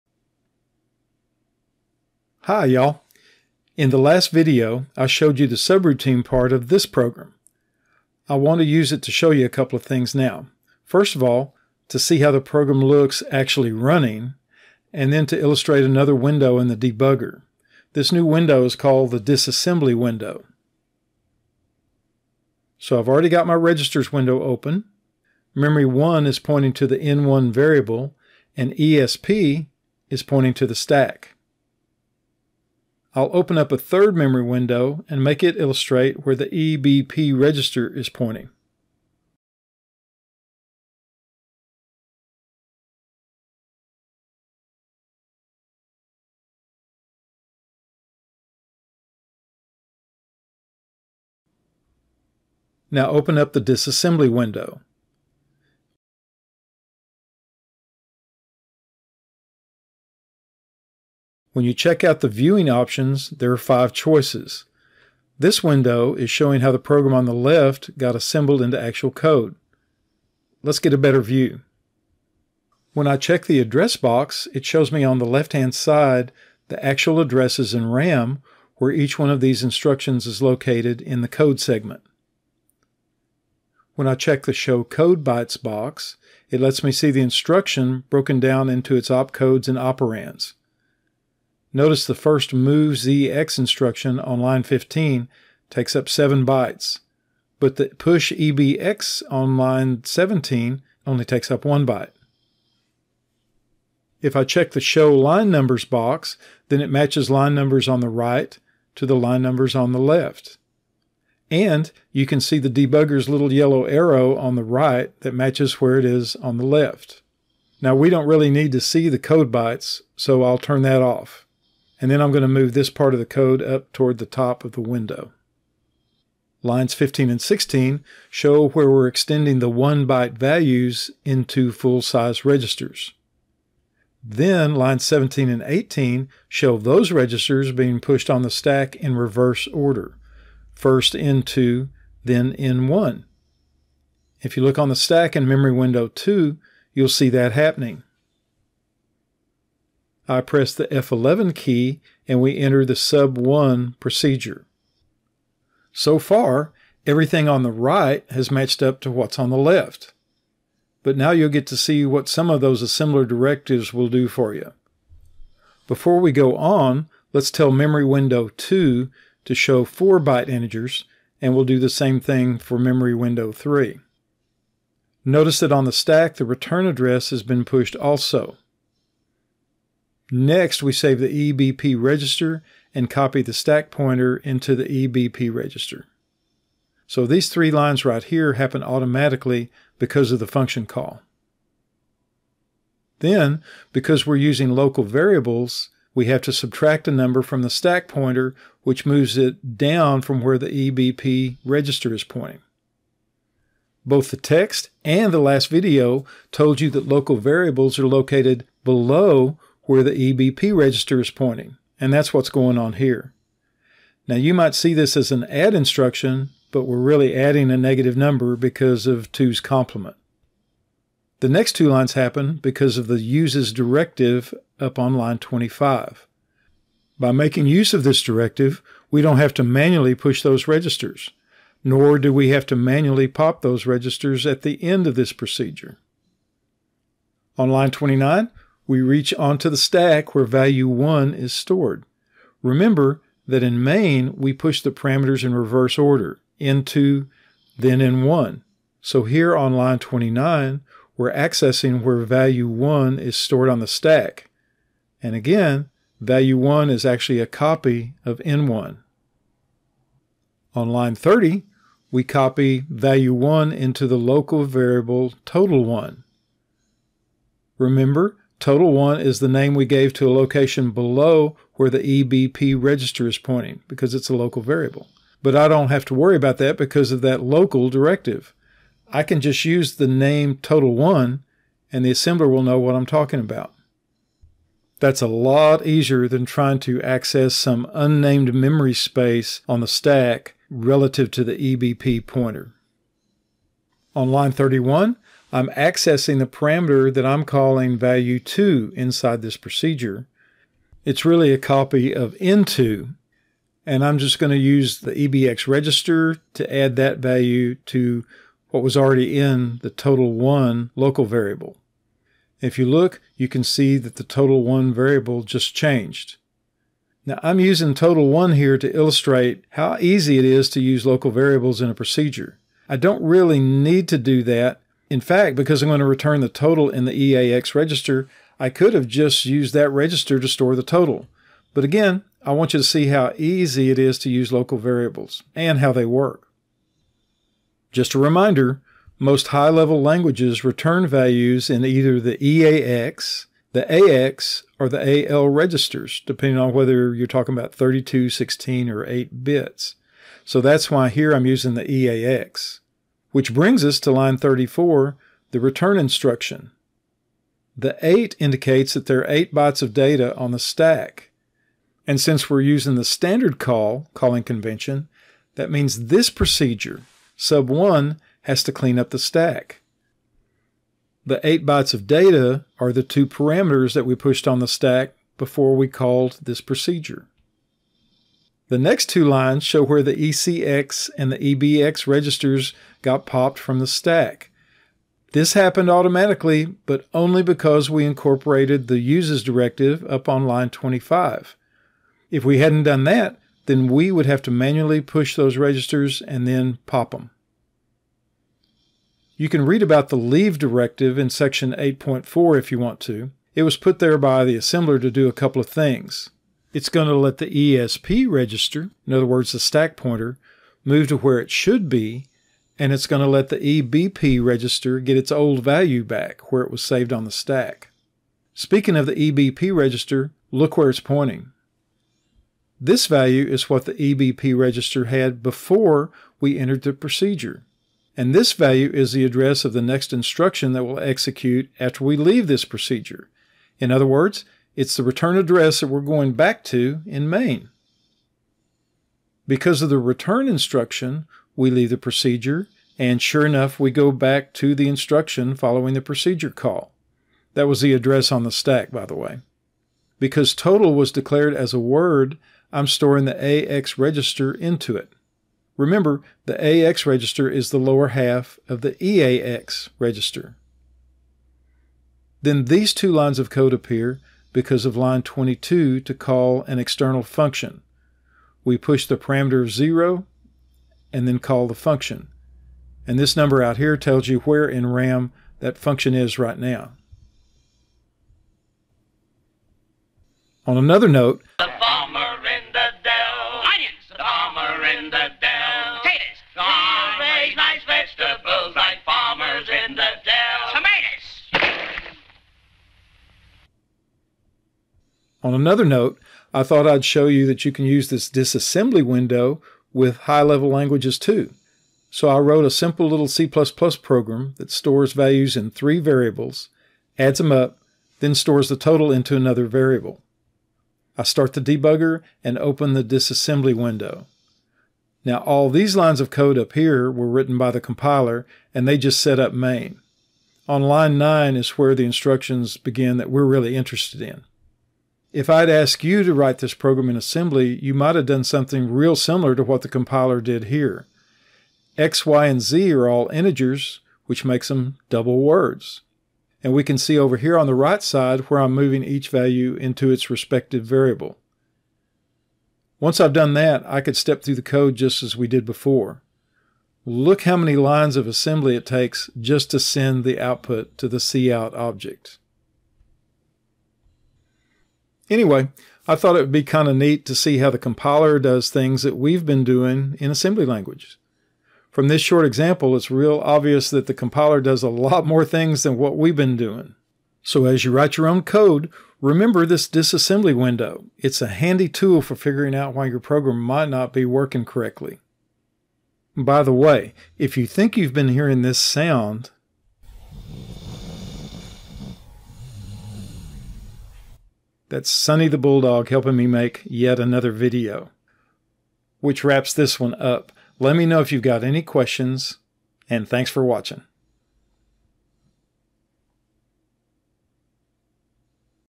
COSC-2425, The Disassembly window
This video shows another debugging tool in Visual Studio. It works for assembly programs and high-level code.